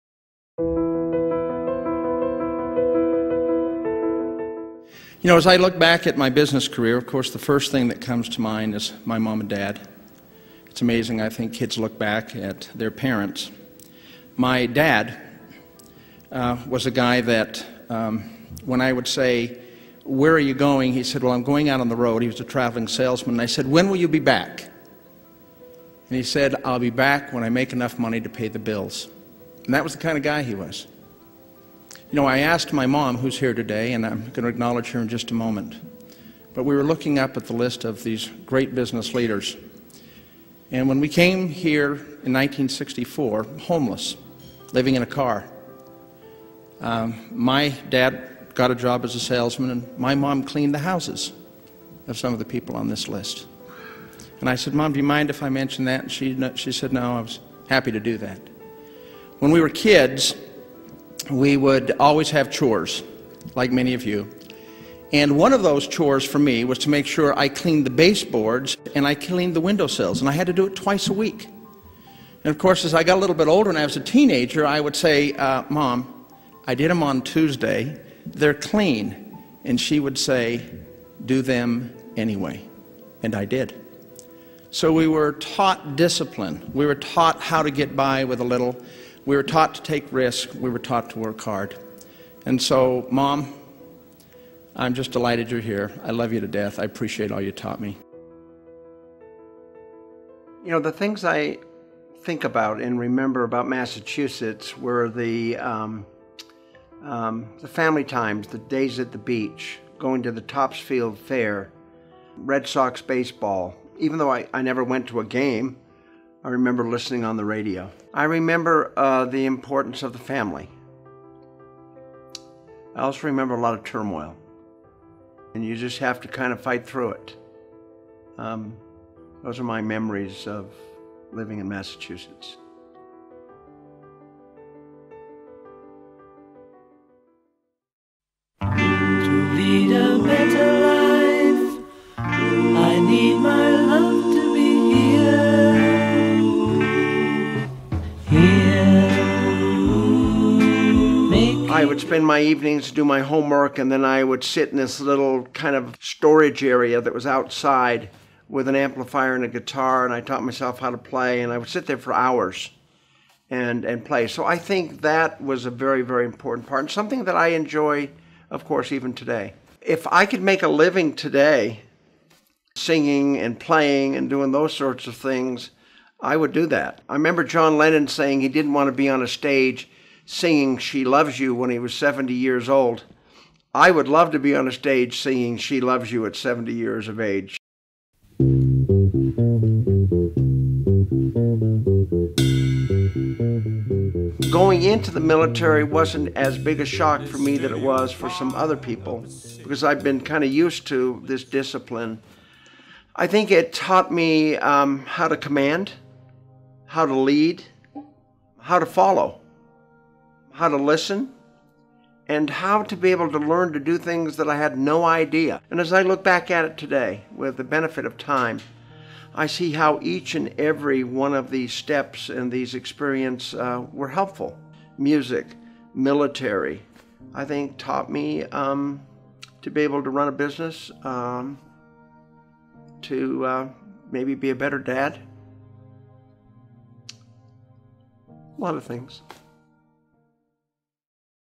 2019 Giant in our City: Fred Lampropoulos, Younger Years
Fred Lampropoulos, chairman and CEO of Merit Medical, was honored as the 40th Giant in our City on May 31, 2019. Watch this video to learn about his younger years.